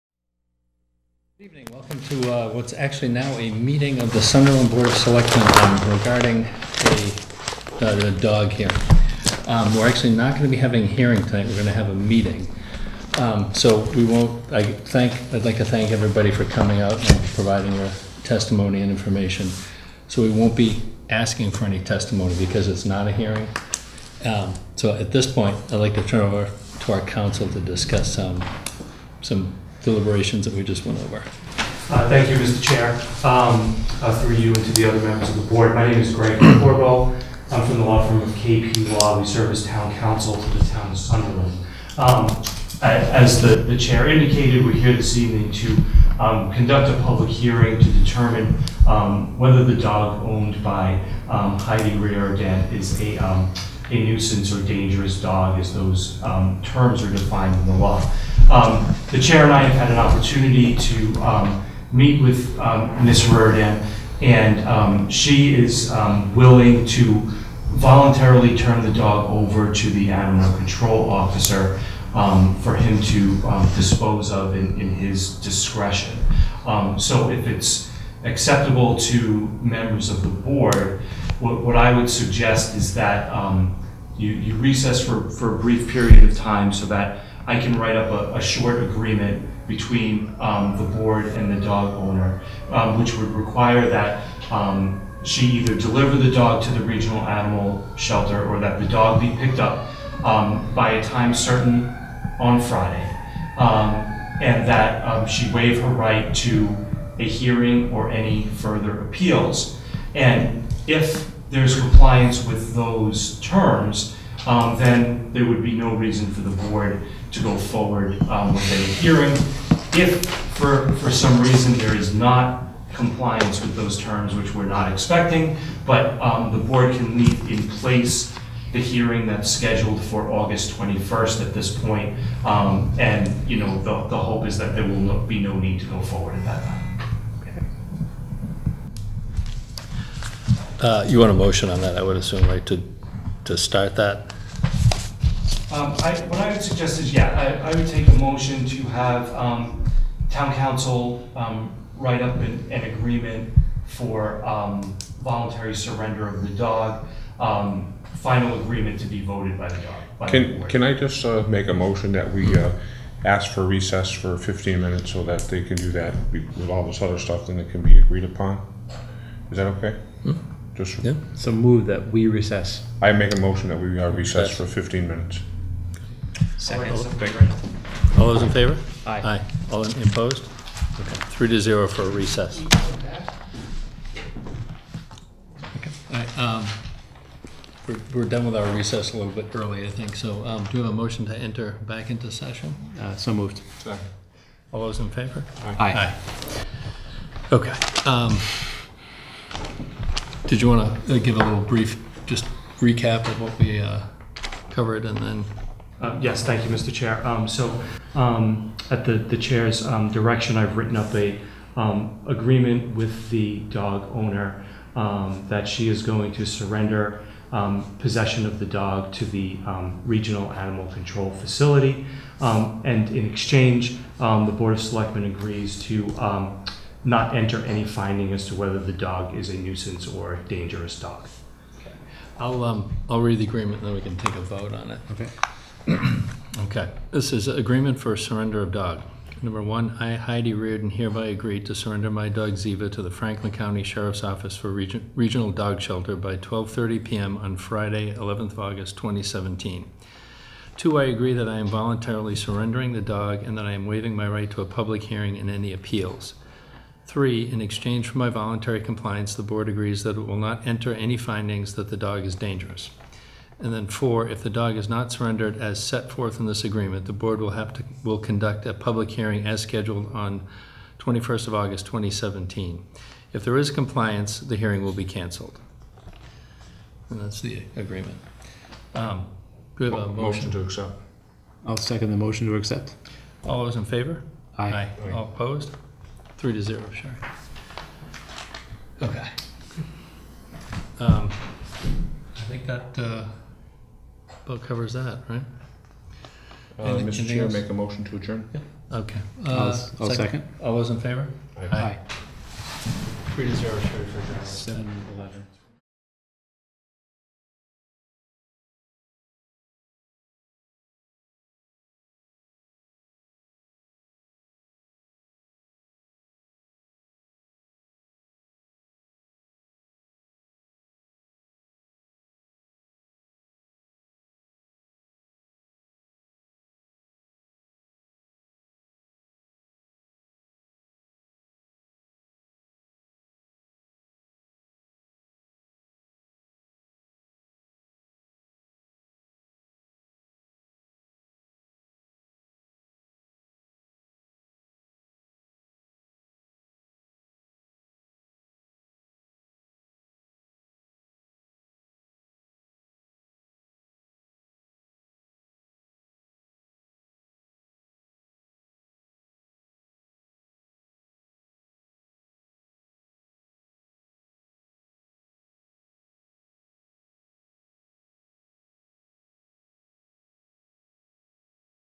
Sunderland Selectboard - August 9, 2017
Meeting of the Sunderland Selectboard for dangerous dog hearing. Recorded August 7th, 2017 at the Sunderland town offices.